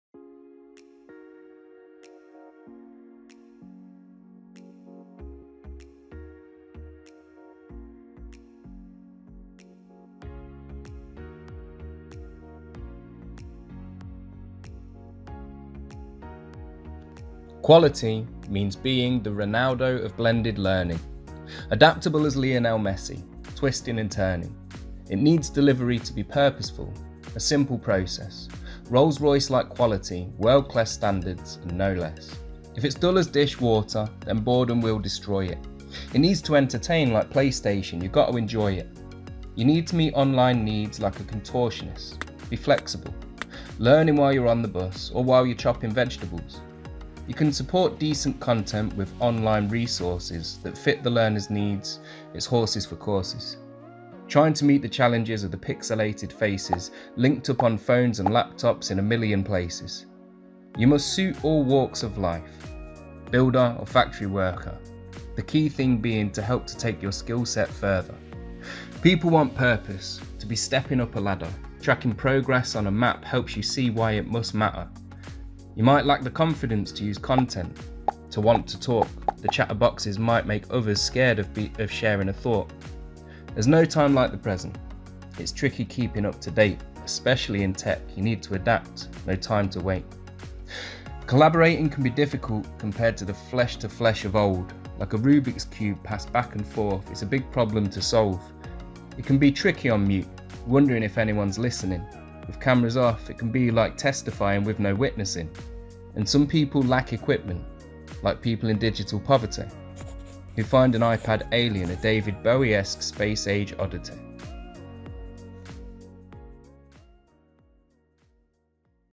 AELP workshop: 'Building quality experiences for learners' - A poem by Andy Craven-Griffiths
Our CEO Mark Dawe recently ran a workshop at the AELP National Conference 2022. The theme was ‘Building quality experiences for learners’ and we asked the audience three questions.

We wanted a creative way to capture the workshop, so we recruited expert creative writer, Andy Craven-Griffiths to work his magic.

Here’s what he had to say…